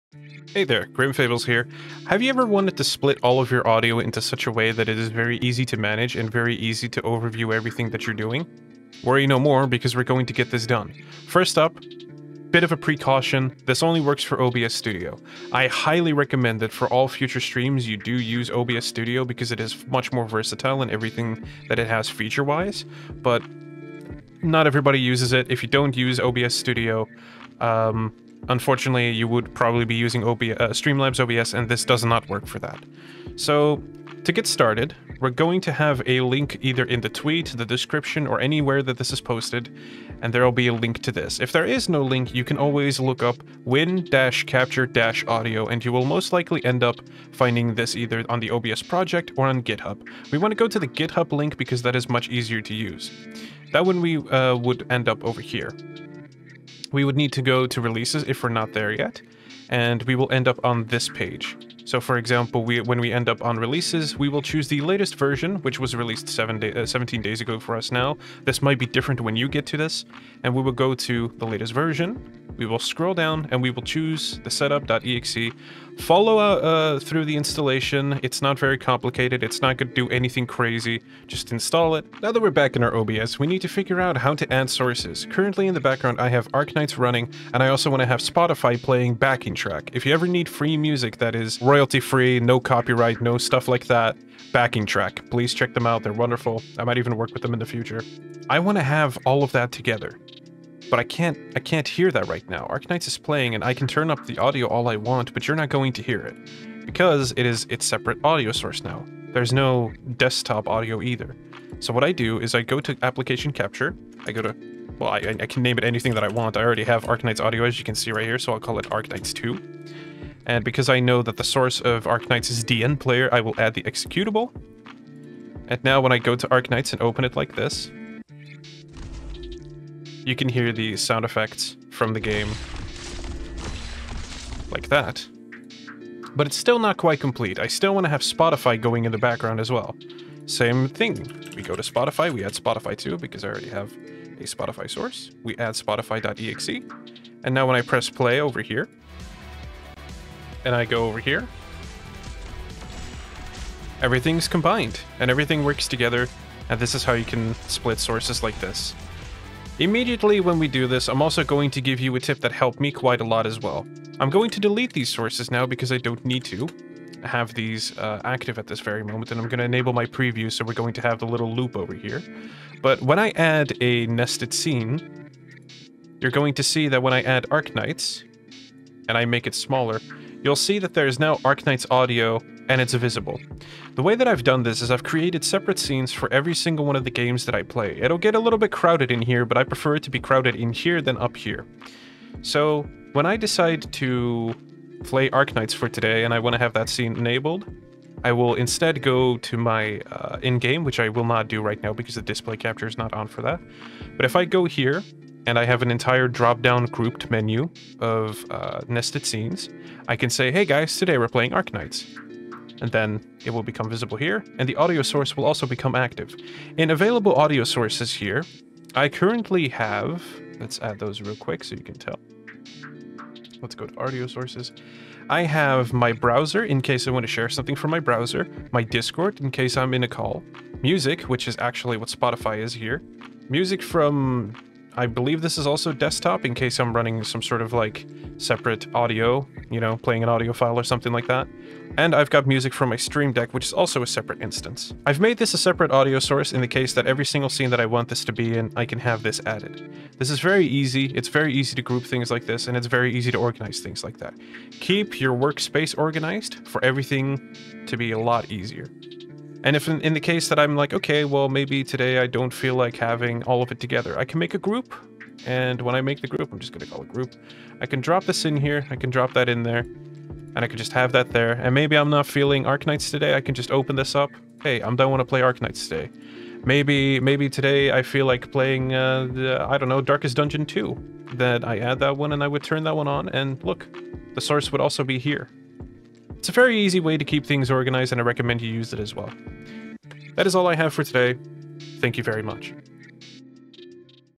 Audio Splitting & Organizing in OBS Studio [2022 Tutorial]
A little tutorial for VTuber & streamer friends.
Here's how I organize my audio & scenes in OBS Studio.